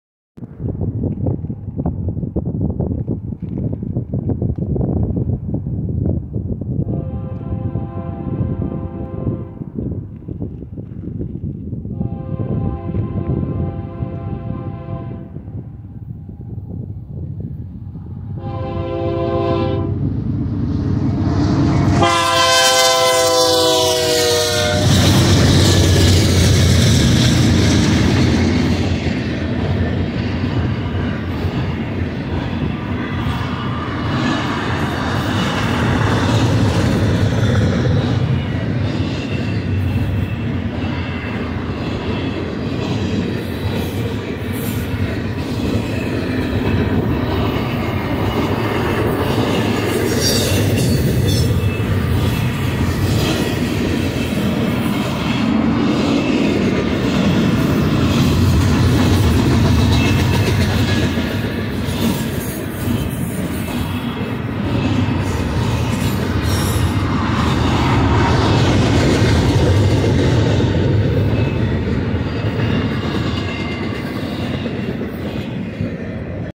WB BNSF Domestic Intermodal Train Feat KCS And Bonnet At Nebo Ca!
This westbound BNSF domestic intermodal train feat KCS and bonnet trail fail came through here at Nebo Ca!

Date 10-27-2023 Time 3:20pm BNSF Train Symbol Q-CHISTO6-25 Corwith, IL-Mariposa Yard - Stockton, CA

Here you will see in this train video is BNSF domestic intermodal train like JB Hunt Intermodal, C.H. Robinson, Amazon Prime, FedEx Multimodal, Martin, ESTES, KLLM Transport, COFC, Walmart. Com, CSX Intermodal How Tomorrow Moves, Prime Intermodal, Double Stack Logistics, R+L Carriers, ABF Freight, Axsun, Highlander Intermodal and Unit 53.com

5433-Dash-9C44-9W (BNSF) (Leading Unit)
4101-SD70ACe (EMD) (KCS) (Trailing)
713-Dash 9-44CW (BNSF) (Bonnet) (Trailing)